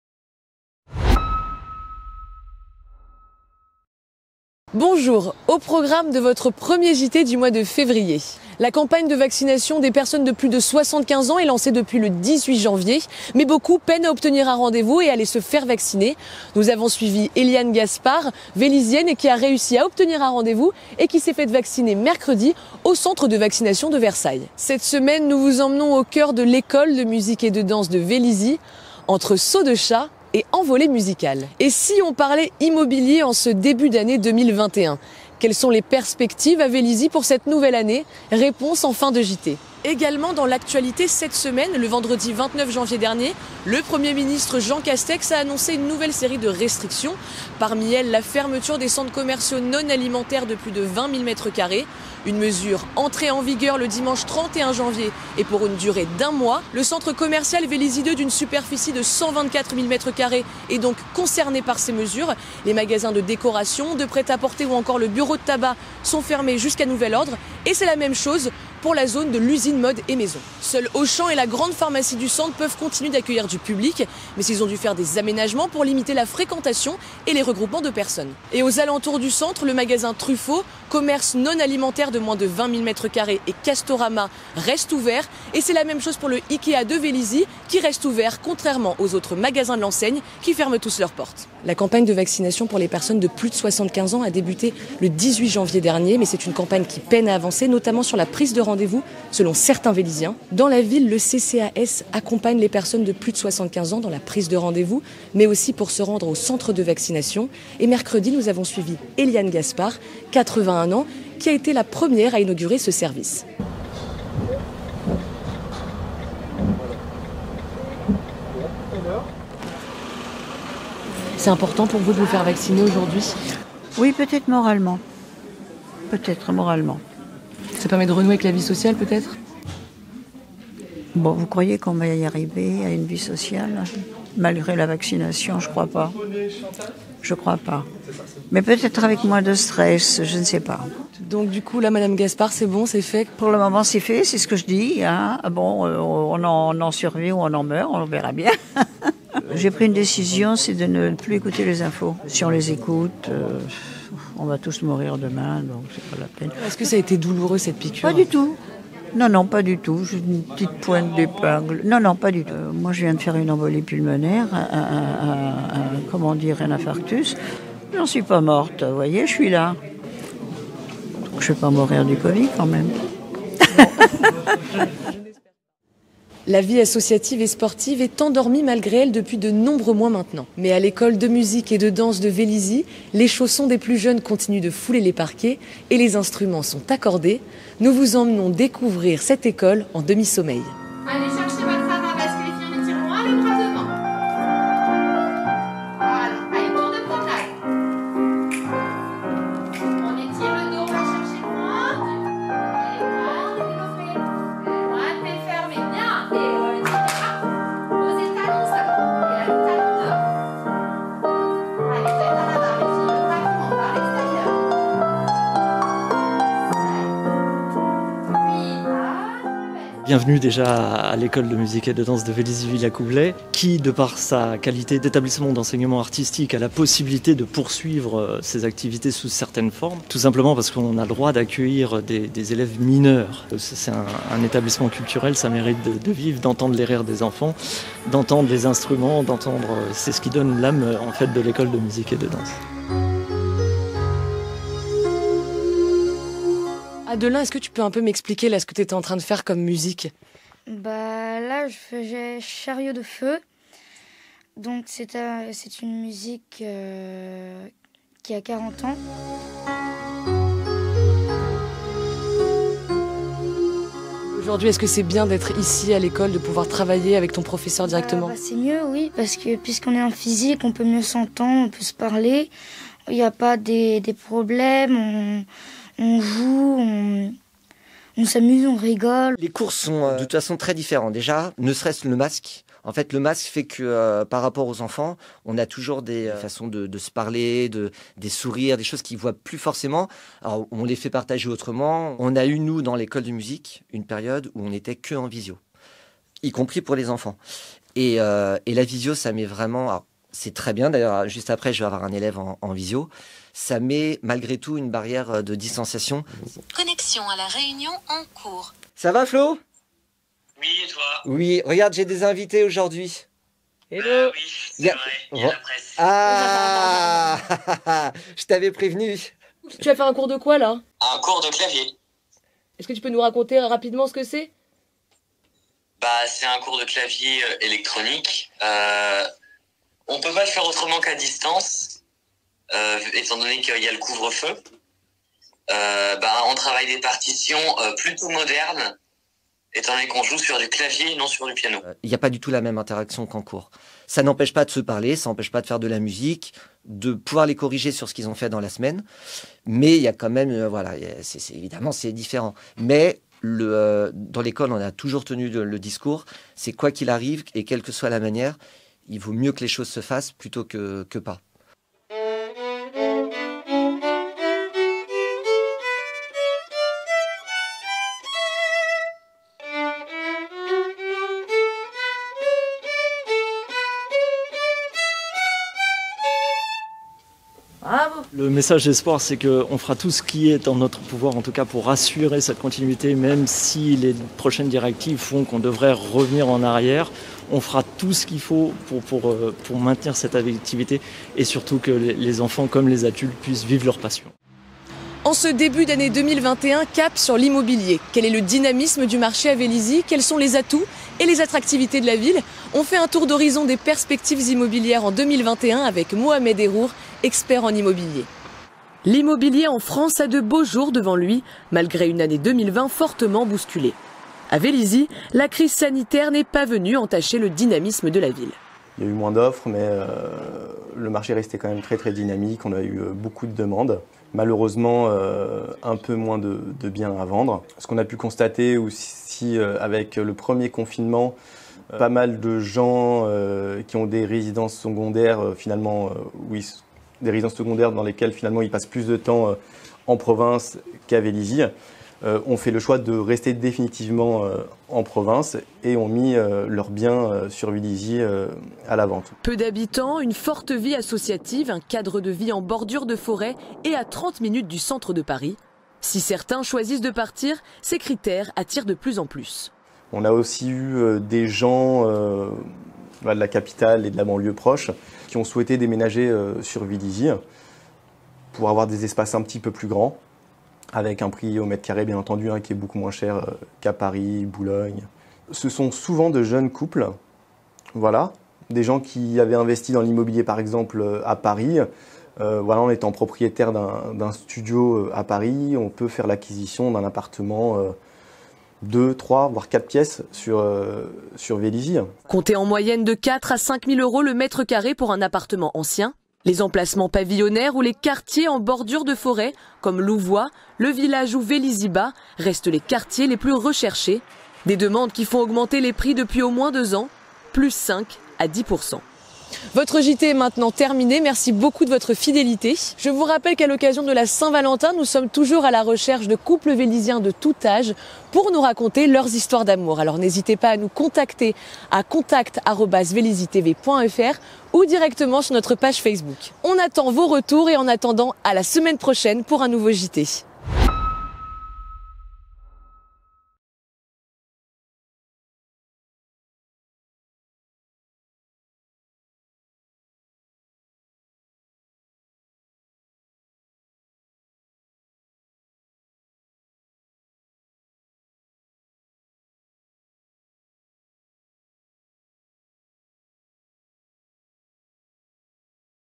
Le JT de Vélizy : 5 février 2021
Au programme de votre JT cette semaine sur Vélizy TV 📺 :

➡️ La campagne de vaccination des plus de 75 ans est lancée depuis le 18 janvier, mais beaucoup peine à se faire vacciner. Mercredi, nous avons suivi Éliane Gaspard au centre de vaccination de Versailles, qui a réussi à obtenir un rendez-vous.

➡️ Cette semaine nous vous proposons un reportage à l’École de Musique et de Danse, entre sauts de chat et envolées musicales.

➡️ Et si on parlait immobilier pour cette année 2021 ? Quelles sont les perspectives immobilières à Vélizy-Villacoublay pour cette nouvelle année ? Réponse en fin de JT.

Merci à tous de votre fidélité ! Bon visionnage !